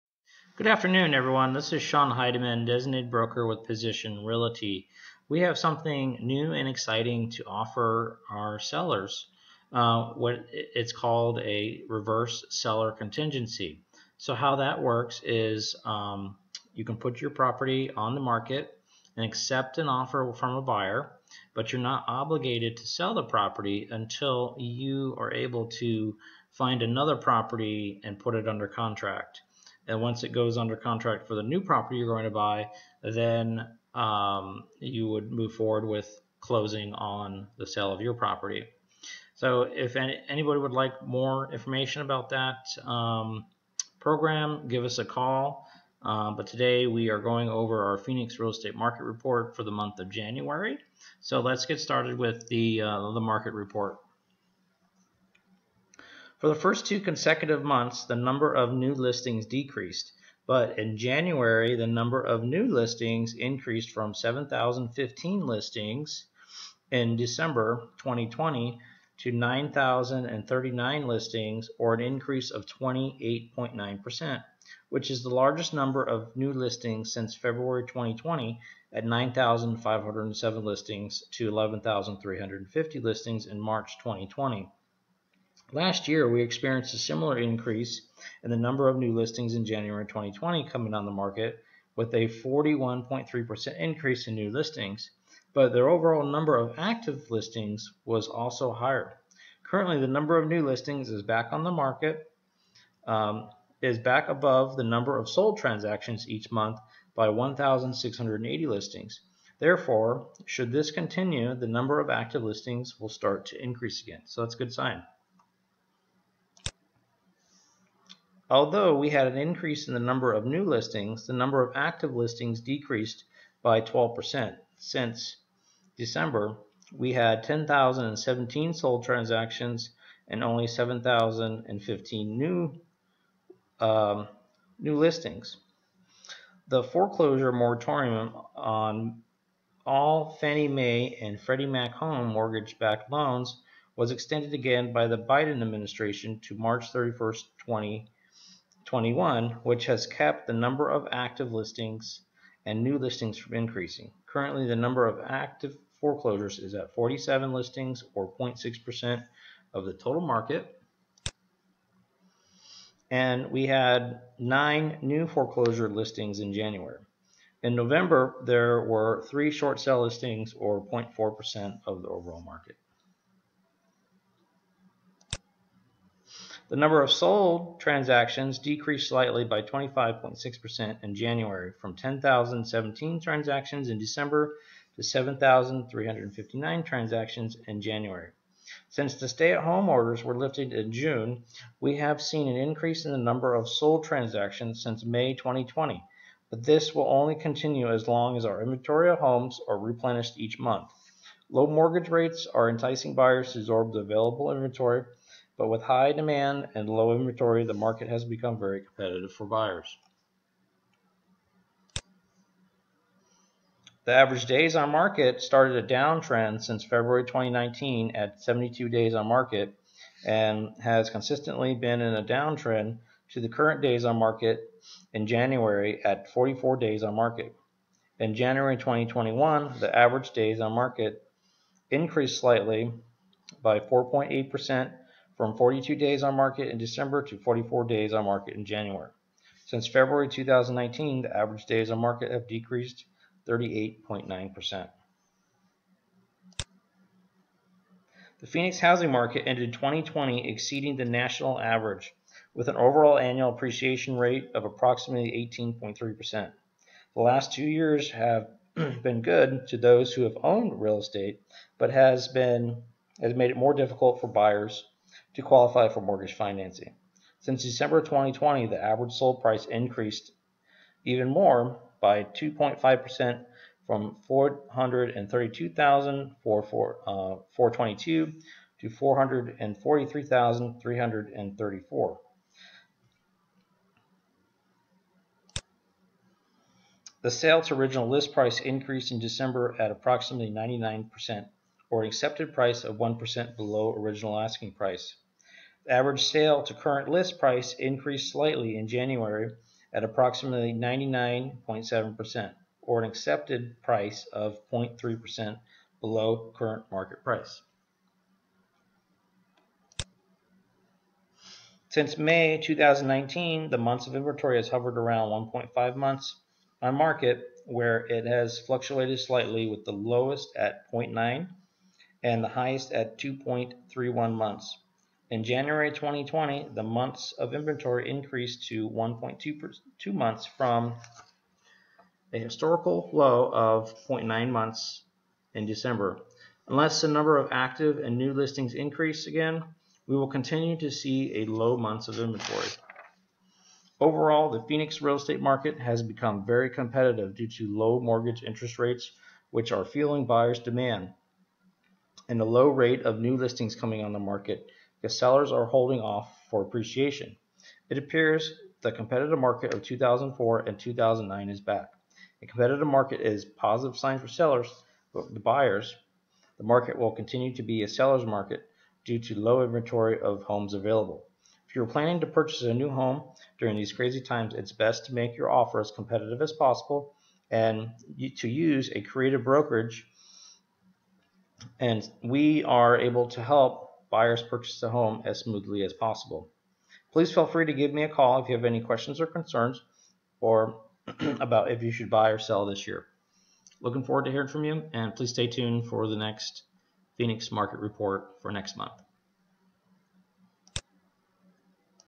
Will Phoenix Finally Start To See Inventory Of Homes Increase?
Watch our monthly Phoenix Real Estate Market Reports to be in the "know" of what's going on in our Phoenix market.